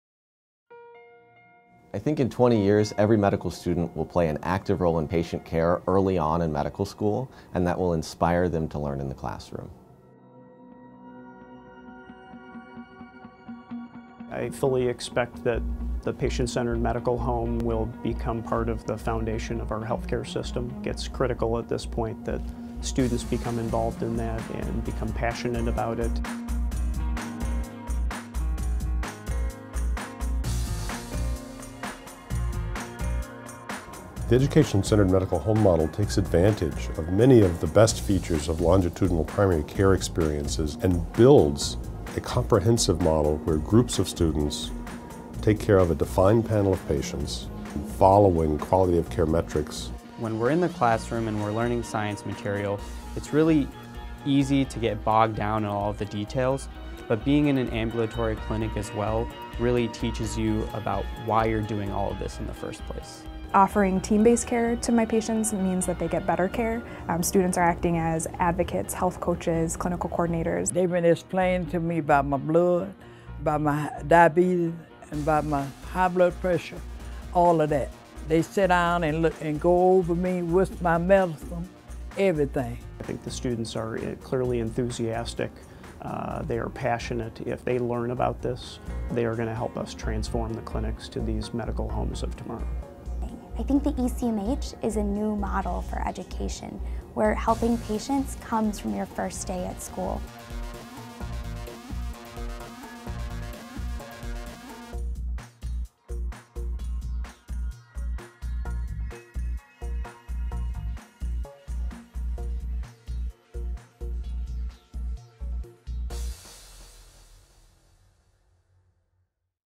Education Centered Medical Home at Feinberg School of Medicine, AAMC Video Submission
What will medical education look like in 20 years? This AAMC video contest submission answers that question. At Northwestern University Feinberg School of Medicine we have created the Education-Centered Medical Home (ECMH), modeled after the Patient-Centered Medical Home, to prepare our students for the practice of medicine by providing them with early and comprehensive educational exposure to team-based care in an authentic outpatient environment, empowering them to take charge of patients longitudinally over their medical school career. To learn more about Northwestern University Feinberg School of Medicine,